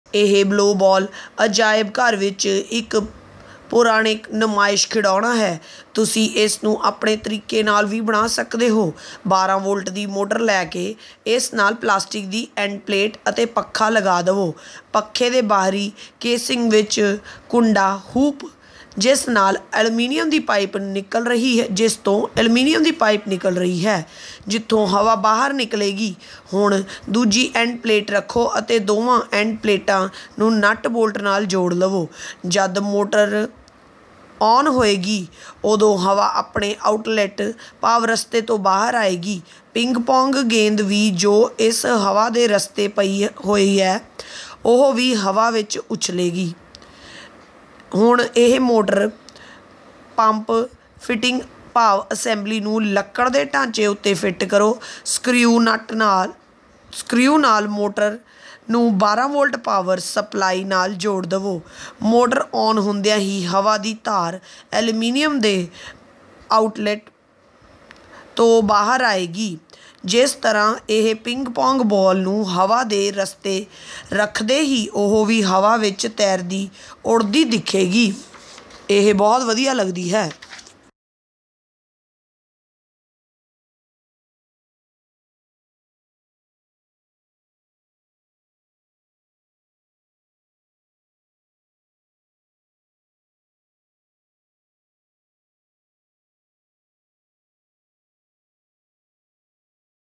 BLOW BALL | Punjabi
This Ball Blower is a standard exhibit in many science museums. You could improvise one of your own. Take a 12-volt motor and attach it to a plastic end plate and a fan. The outer casing of the fan is a hoop with an aluminum pipe jutting out for the outlet of air. Place the other end plate and secure both the end plates with long bolts and nuts.  When you switch ON the motor a stream of air will come out of the outlet. A ping-pong ball placed on the tube will float in the air. Mount this motor-pump assembly on a wooden stand with screws. Connect the motor to a 12-volt power supply. After switching ON the motor air will gush out of the aluminum outlet. Place a ping-pong ball in the air stream and it will float in the air stream.